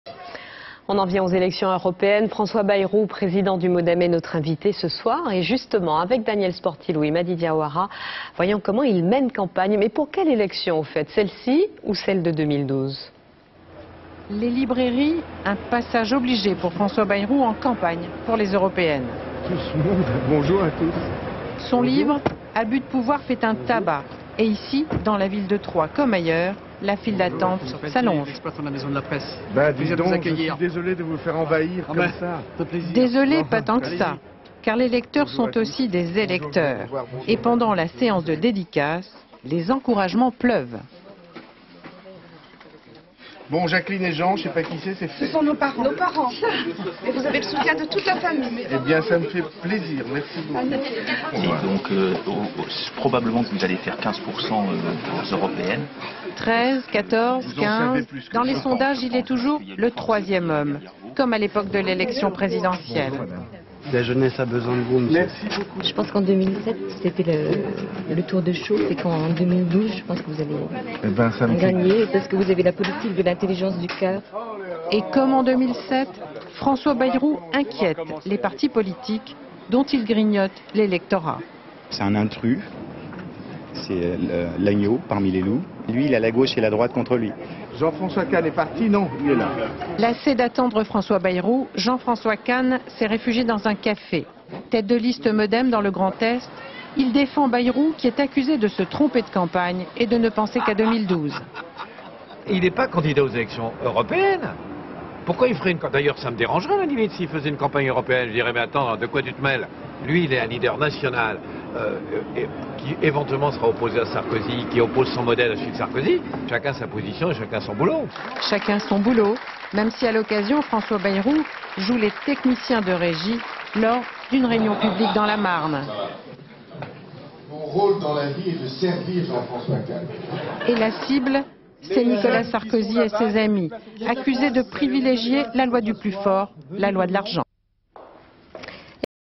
Elections européennes : la campagne électorale de François Bayrou
3 juin 2009
Reportage à Troyes (Aube), consacré à la campagne électorale de François Bayrou à l'occasion des élections europénnes. Interview de Jean François KAHN, tête de liste Modem, sur François BAYROU. Commentaire sur images factuelles.     François BAYROU s'est rendu à Troyes pour soutenir le candidat du Modem dans la circonscription Est aux prochaines élections européennes, Jean François KAHN. Il en a profité pour se livrer à une séance de dédicace de son dernier livre "Abus de pouvoir". De quoi faire dire à ses adversaires qu'il se concentre avant tout sur l'élection présidentielle. Images d'archive INA